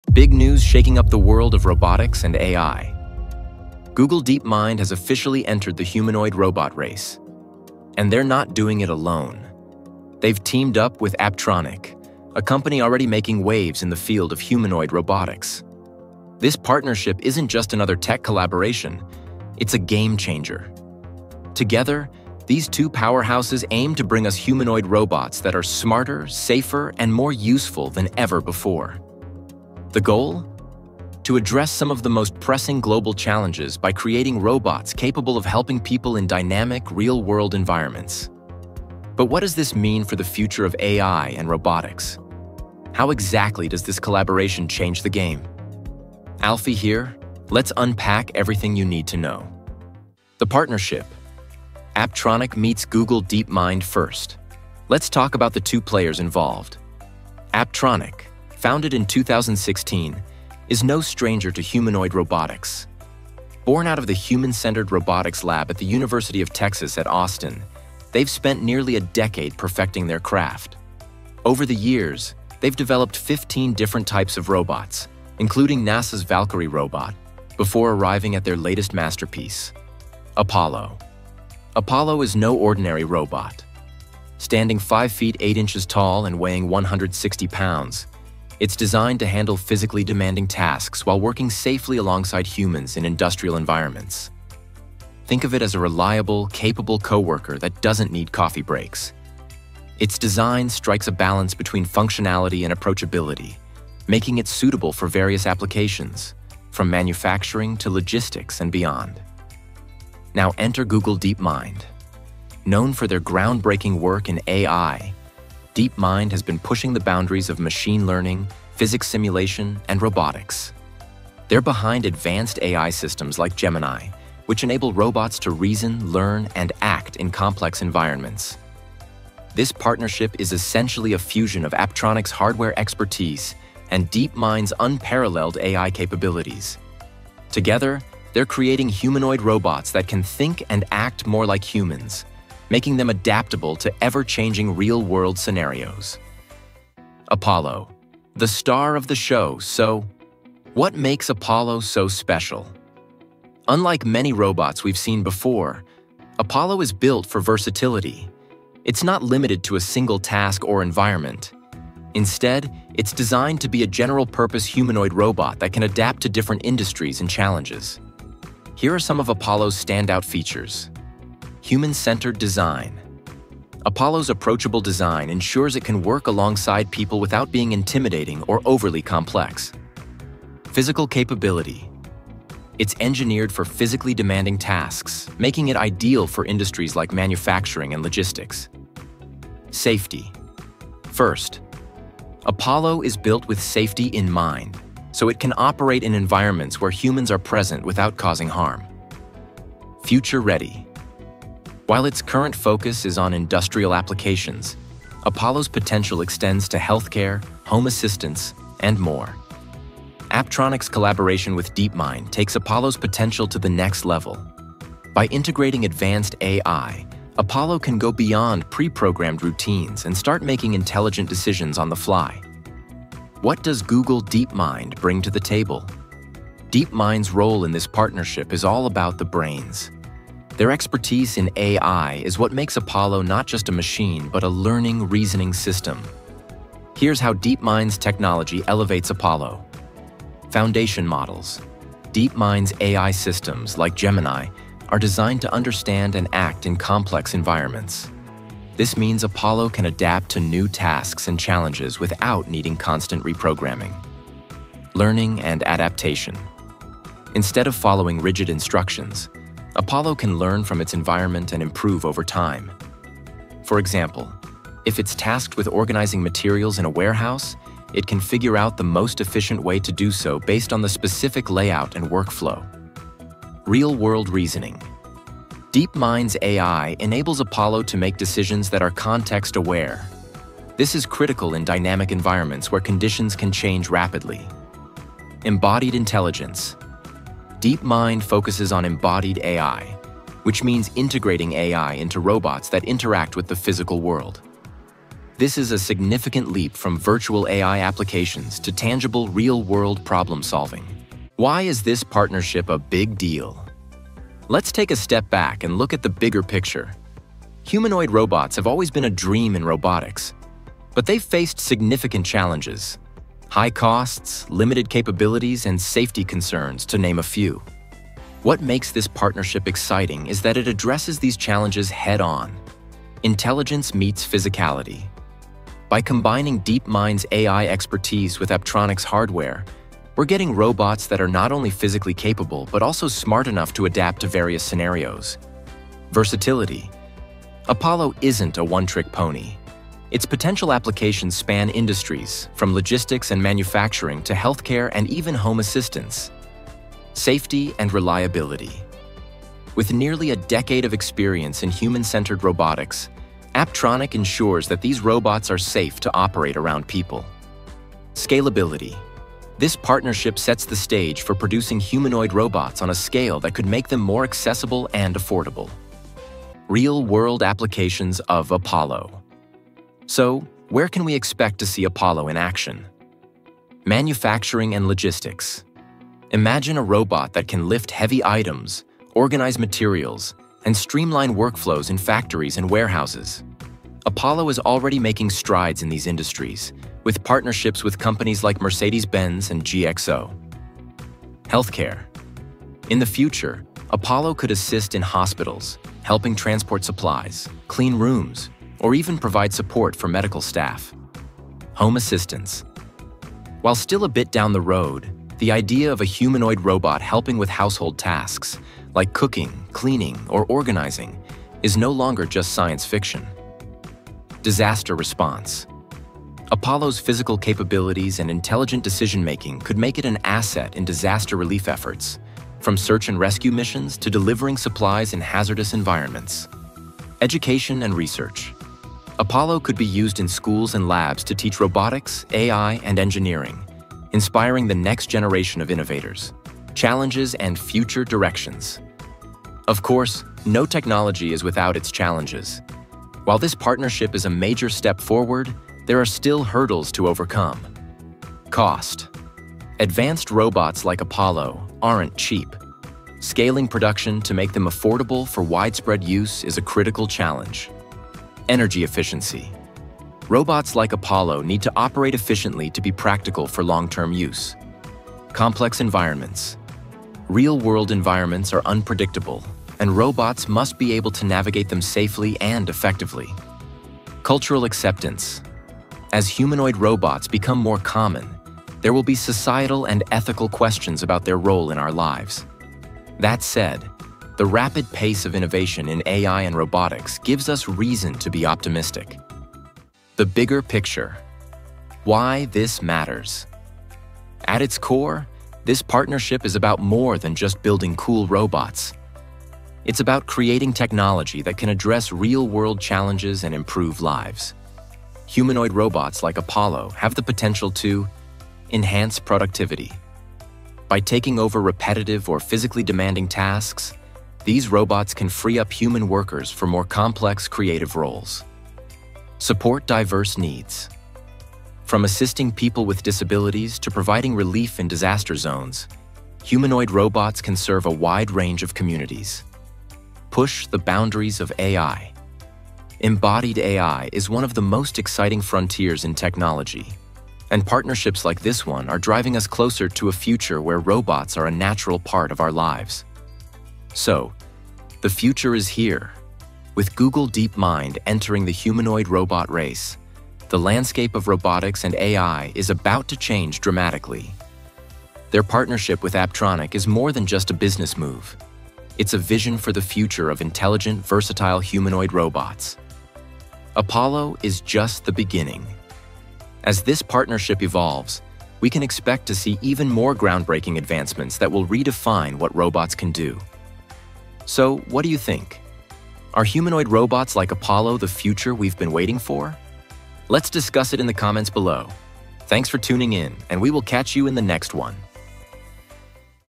MASSIVE UPDATE! Google Deepmind Enters Ai Humanoid Robot Race!
🚨 Breaking News! 🚨 Google DeepMind has just entered the AI humanoid robot race, marking a monumental leap in artificial intelligence and robotics! 🤖💥 Imagine a world where machines not only think but move and interact like humans. This groundbreaking development could redefine technology and human interaction as we know it. 🌐